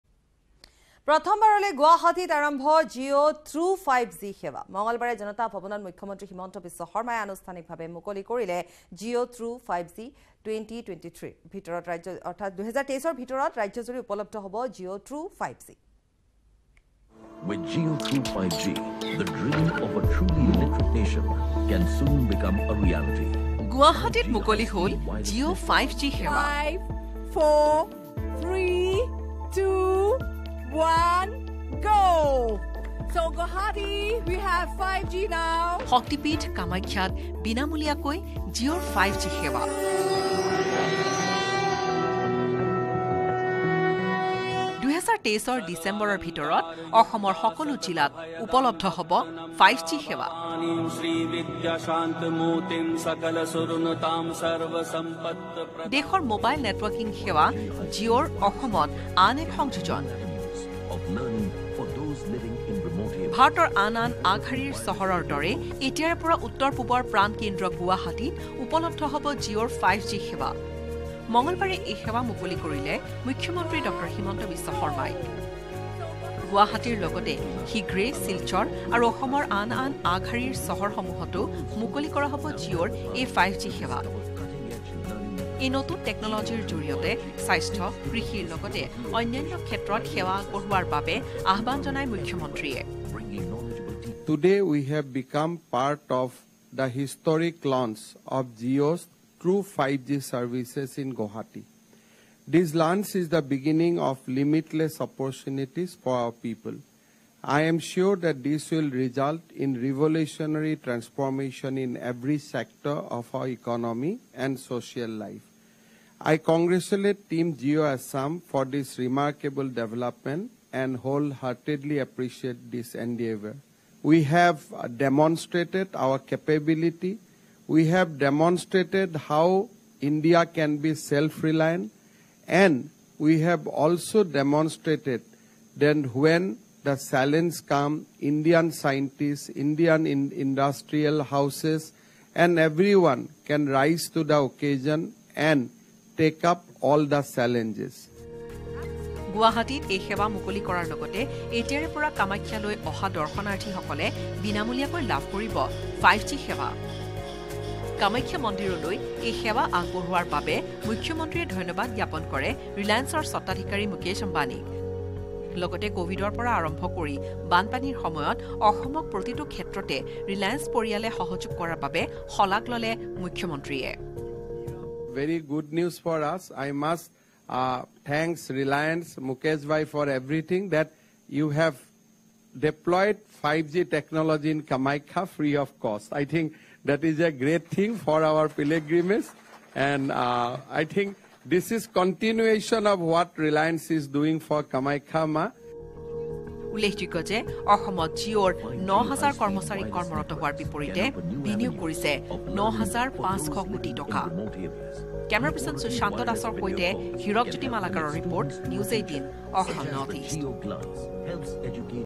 Jio Connects With Free Wi-Fi at Kamakhya Temple : কামাখ্যাত লাভ কৰিব জিঅ'Wi-Fi সেৱা। Assamese News
Jio Connects With Free Wi-Fi at Kamakhya Temple । কামাখ্যাত দৰ্শনাৰ্থীয়ে লাভ কৰিব জিঅ ট্ৰু 5G সেৱা